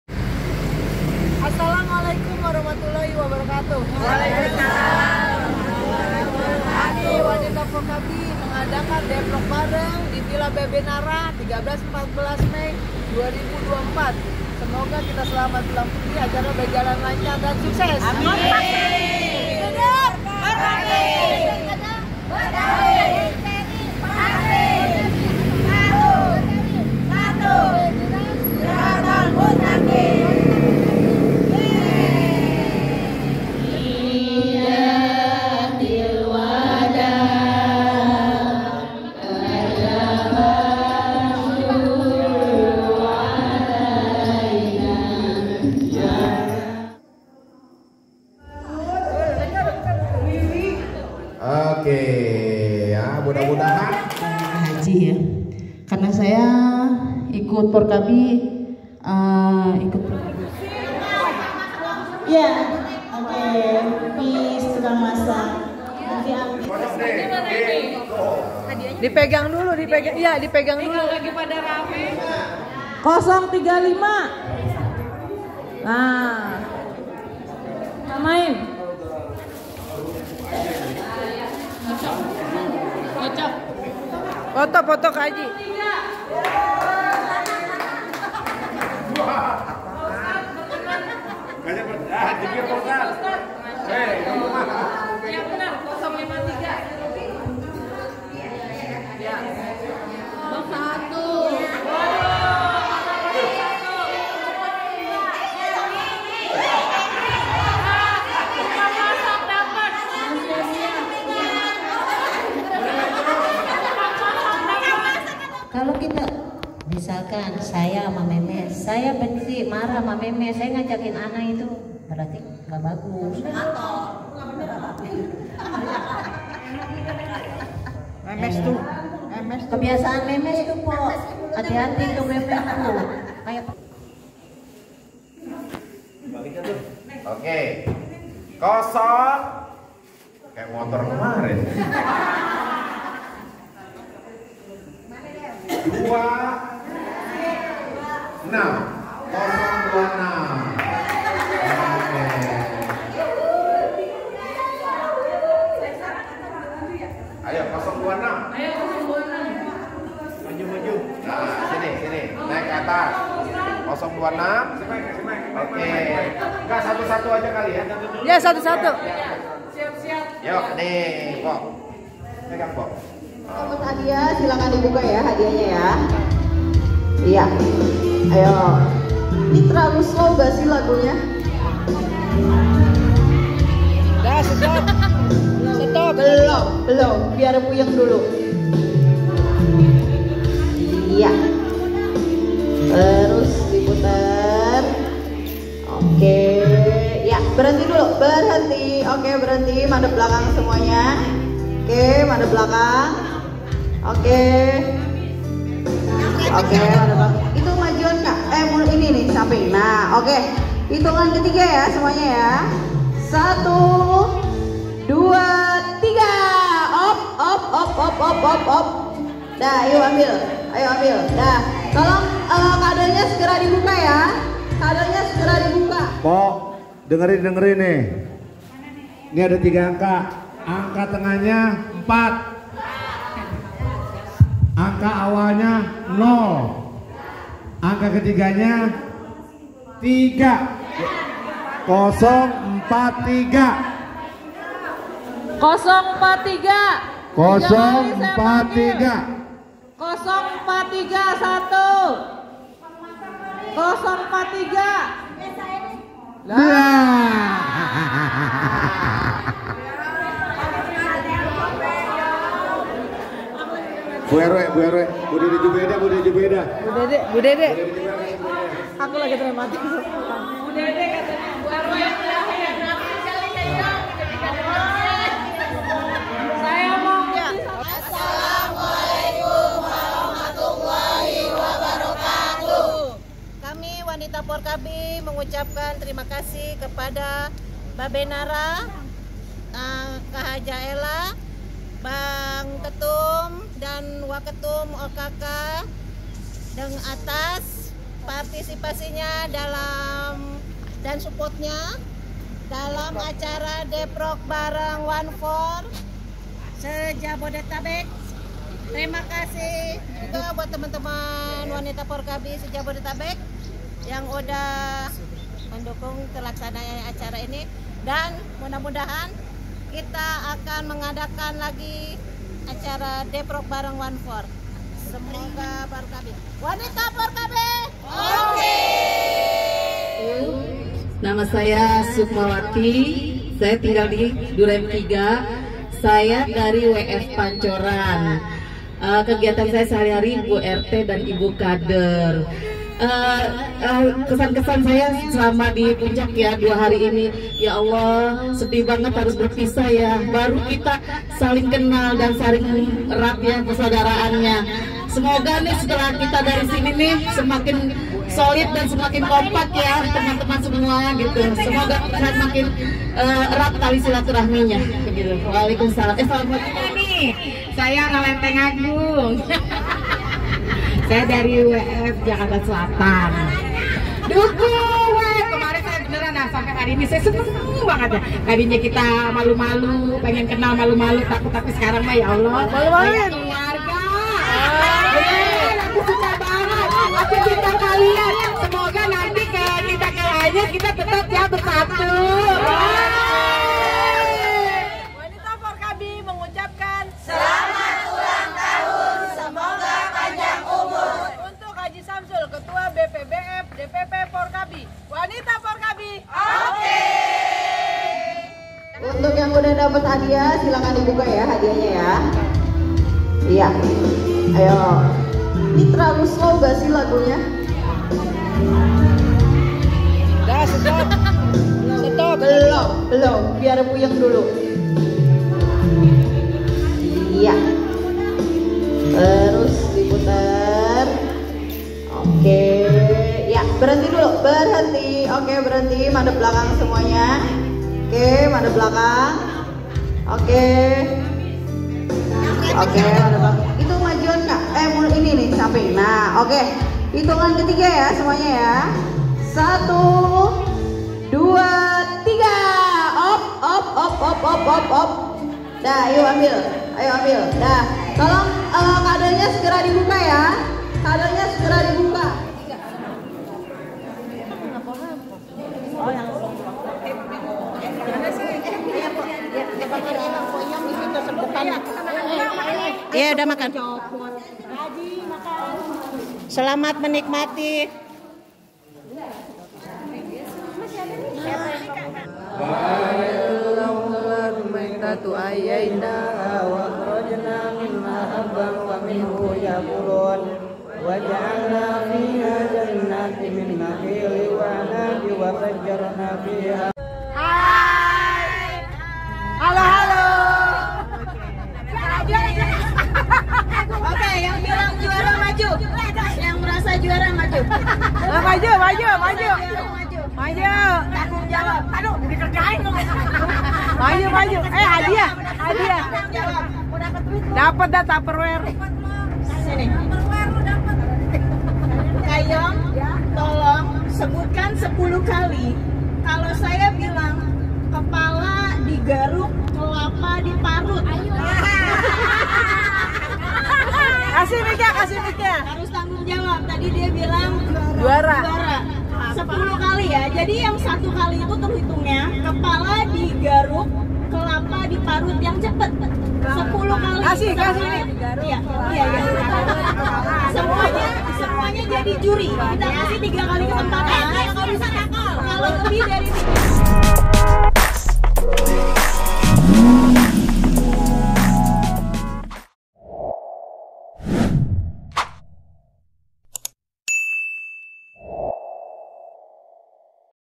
DEPROK Bareng Wanfor FORKABI Puncak Villa 13-14 Moga Tetap Terjalin Silaturahmi
Viral DEPROK bareng wanfor puncak villa 13_14  moga ttp terjalin silaturahmi yg harmonis dan menambah ke solidan antar anggota wanfor se-jabodetabek, terimakasih buat babeh Mayjend purnawirawan babeh Nara,  bng ketum, bng Waketum, Mpo hj Ella ,dan smua para wanfor yg adh mendukung acara dari kita untuk kita,